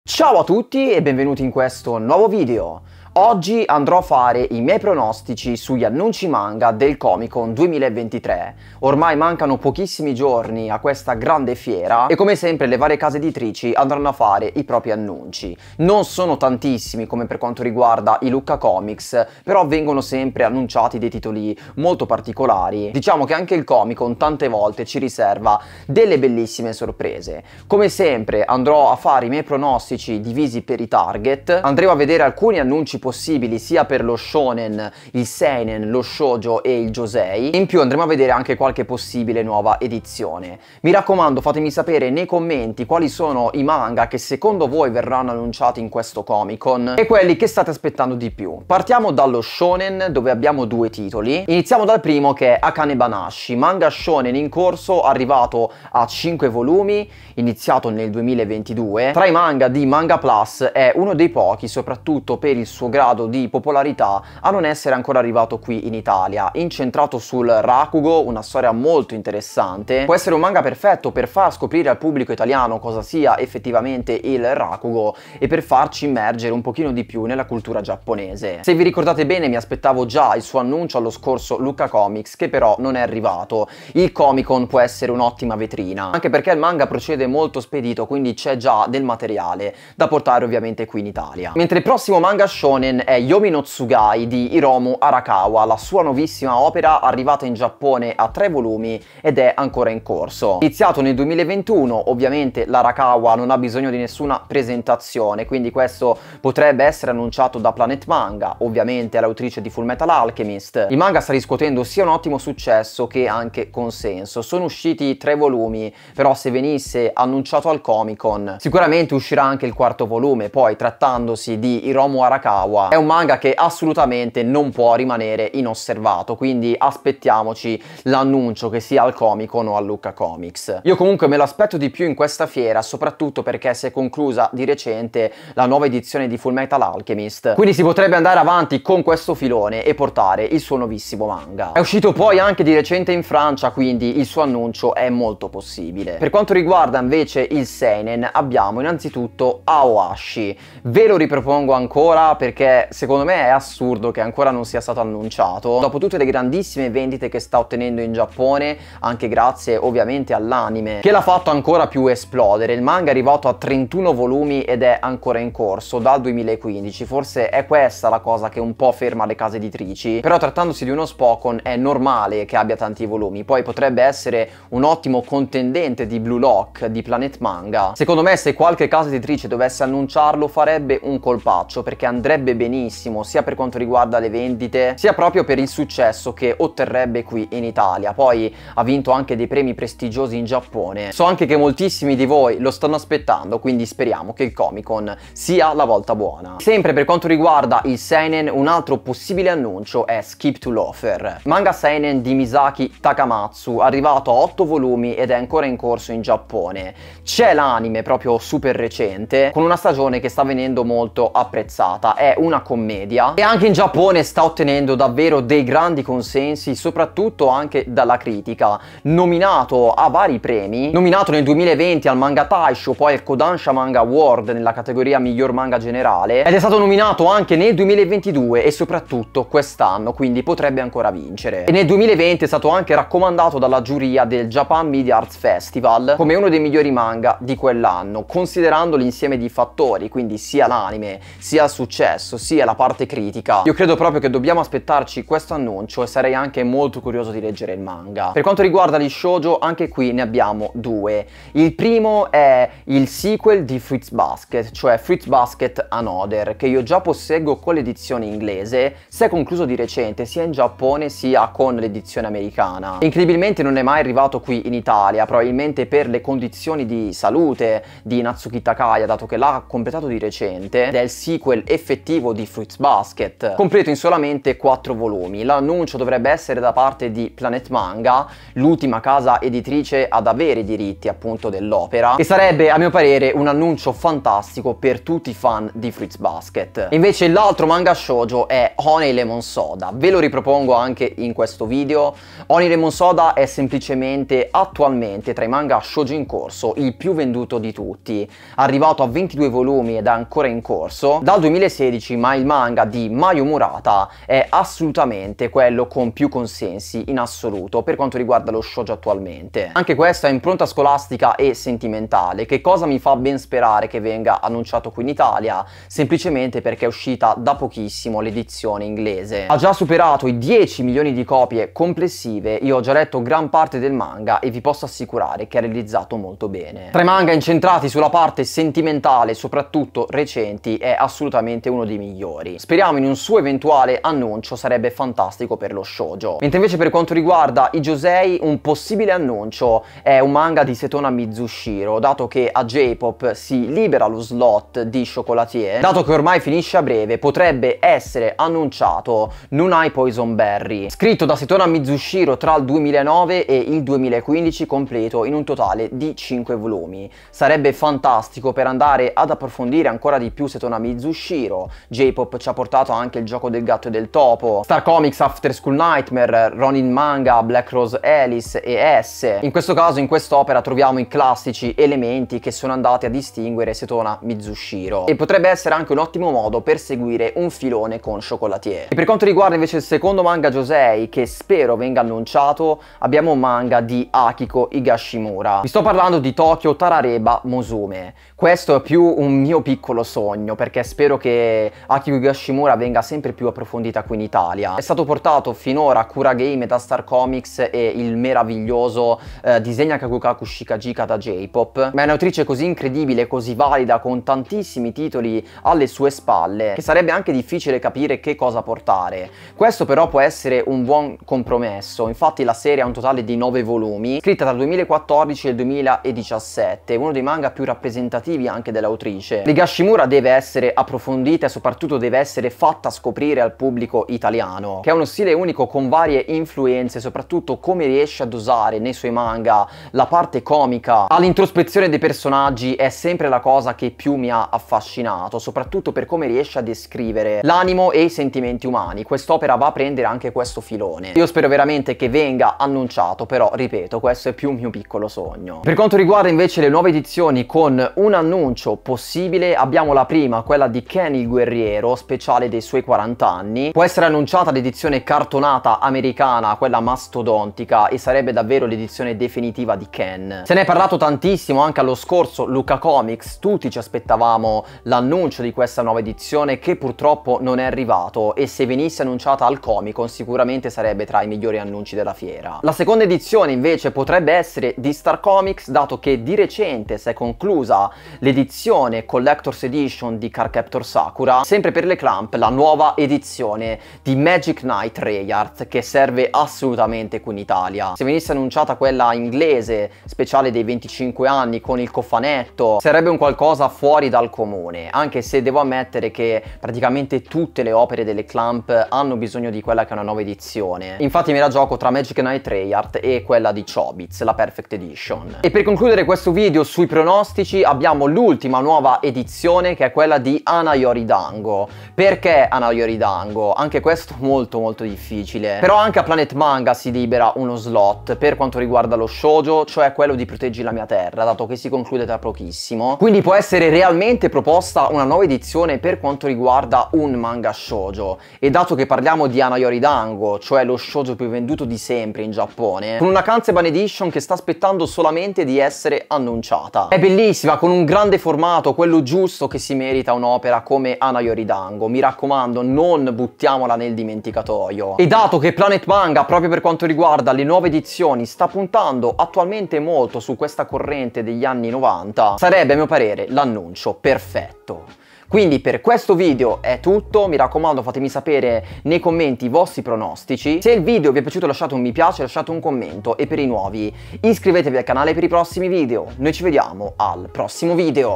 PRONOSTICI ANNUNCI MANGA COMICON 2023
Nuovo video dedicato ai miei pronostici degli annunci manga del Comicon 2023! Fatemi sapere cosa ne pensate e quali voi state aspettando :)